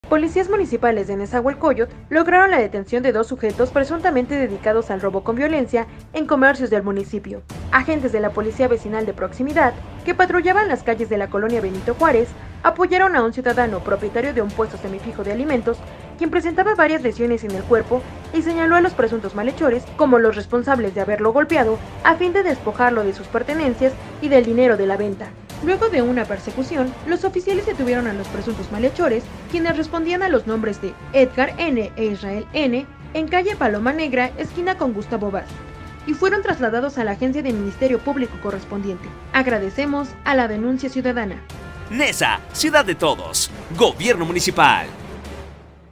Detienen policías municipales de Neza a par de peligrosos asaltantes
Elementos de la policía municipal de Nezahualcóyotl lograron la detención de dos sujetos presuntamente dedicados al robó con violencia a comercios.

Ante cualquier emergencia comunícate al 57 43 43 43